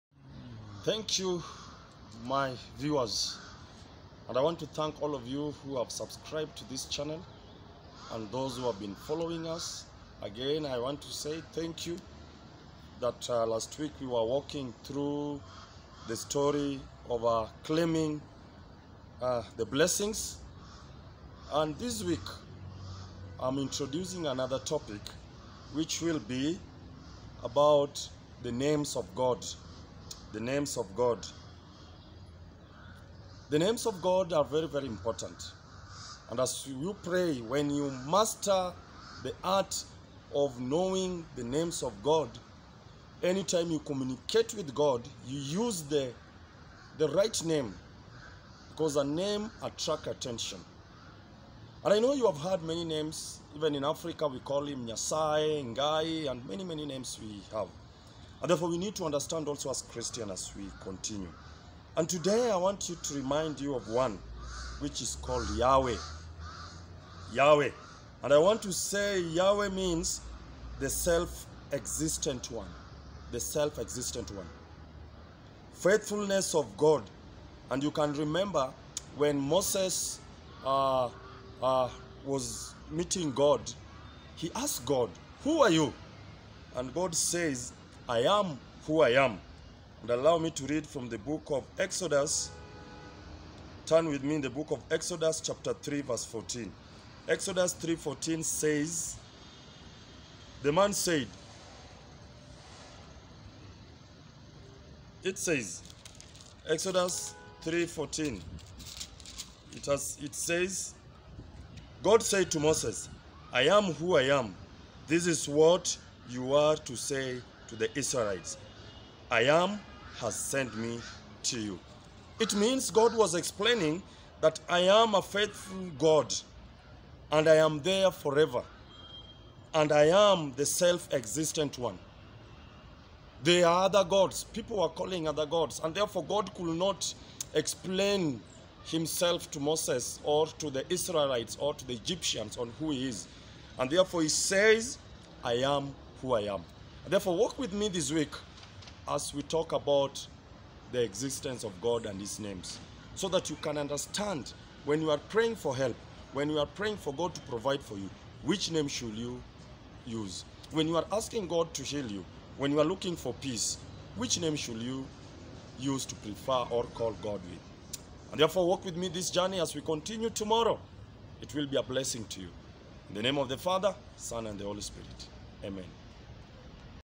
Introduction to Names of God
We need to understand that the biblical names of God have a great significance